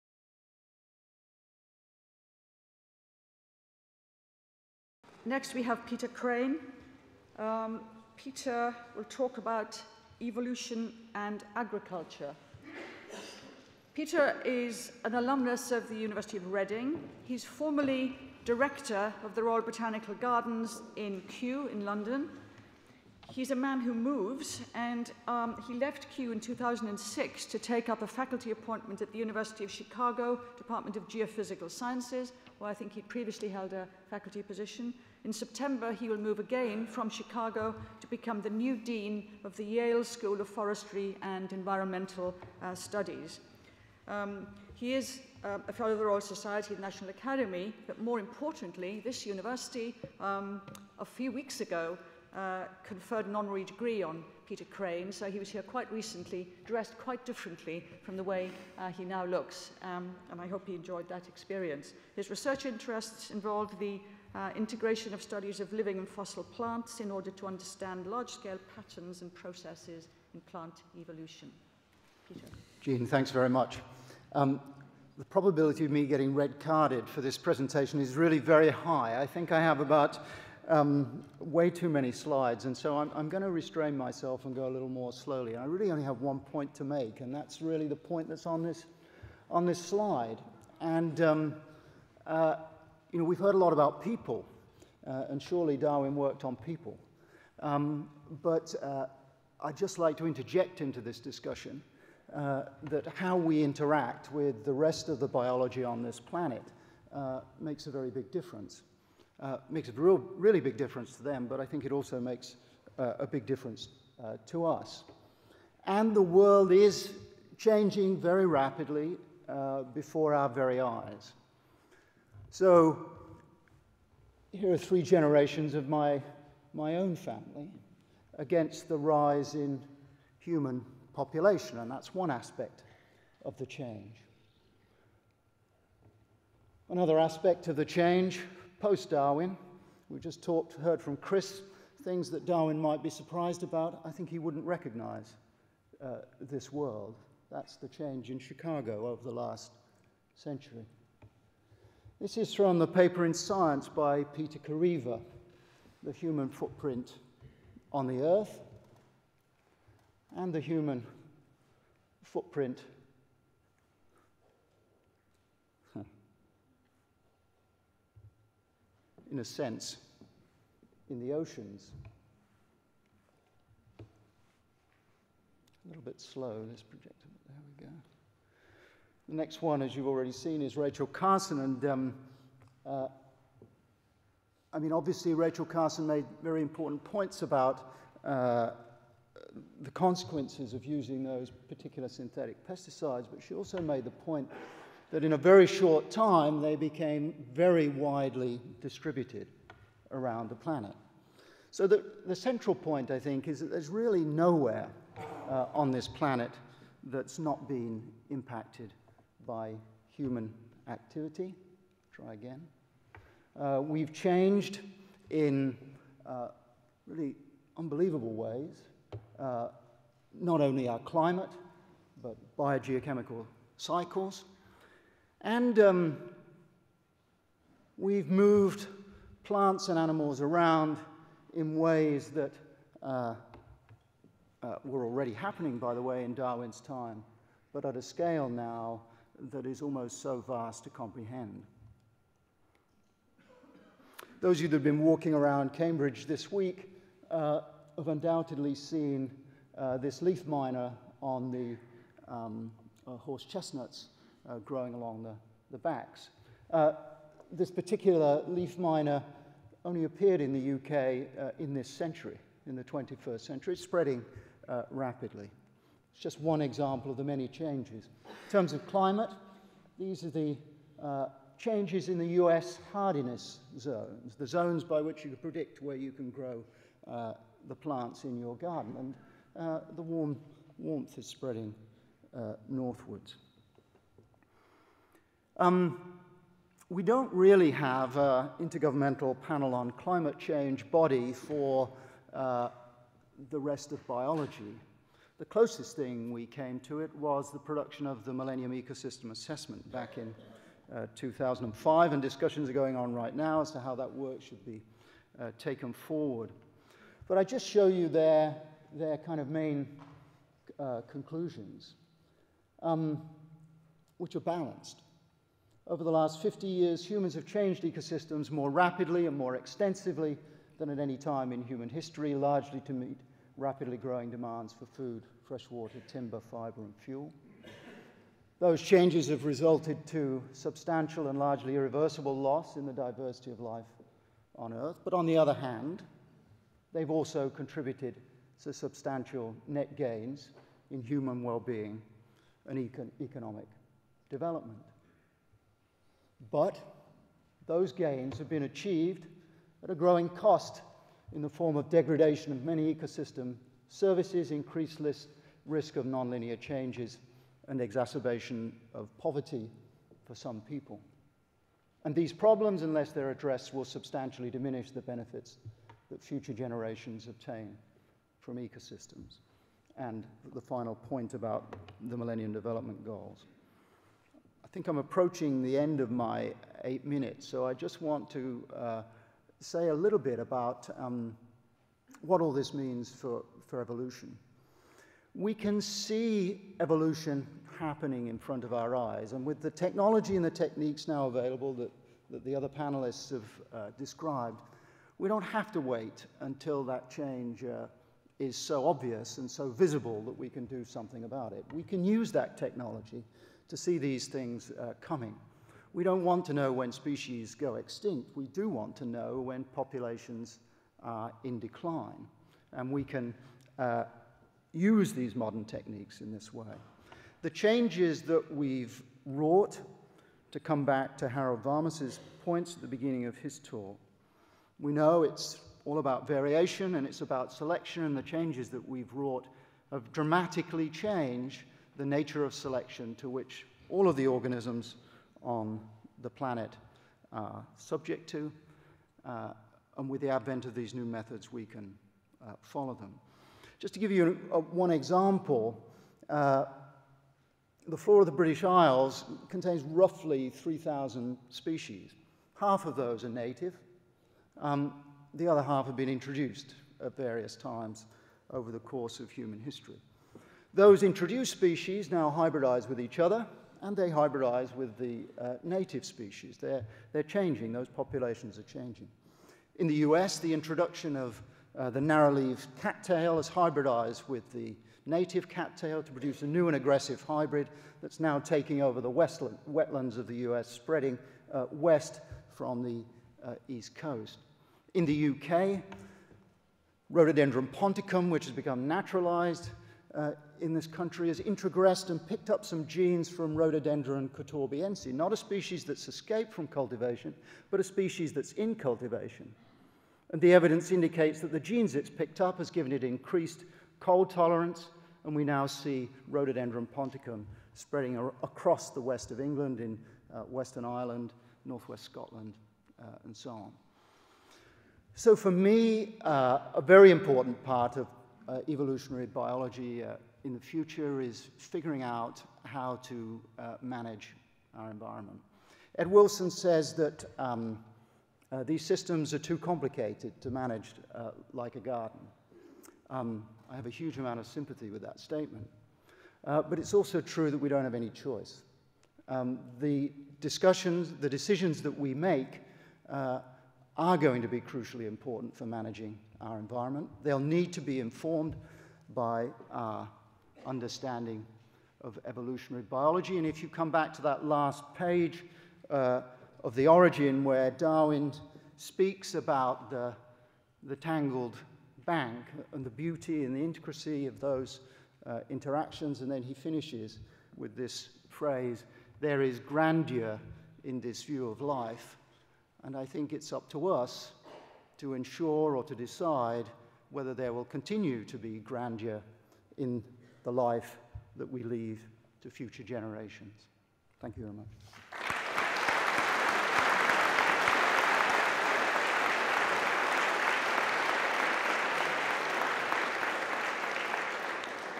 Sir Peter Crane, Darwin and modern science, Thurs 9 July
chicago, The importance of trees: recent progress with understanding the history of plant life
Professor Sir Peter Crane (University of Chicago, Illinois, USA)

Summary: Much was learnt about plant evolution in the first 100 years following On the Origin of Species, but progress in recent decades has been equally rapid, especially with new approaches to develop and test different kinds of evolutionary trees that model the specific pathways of plant evolution. Such trees provide a basis for understanding how the major groups of living and fossil plants are interrelated, and in turn, this has opened up possibilities for research in many new areas. In this lecture, I will review our current understanding of the origin and early diversification of land plants, vascular plants, seed plants and flowering plants. In all four cases, many questions remain to be answered, but improved insights have come, most reliably, by integrating information from living and fossil plants towards the development of increasingly robust phylogenetic and stratigraphic patterns. Among living plants, the widespread application of phylogenetic techniques based on molecular sequence data, together with new studies of plant structure, function and development, have been especially influential. Among fossil plants, new perspectives continue to come from studies of recently discovered and classic localities, as well as from information obtained through the application of new techniques.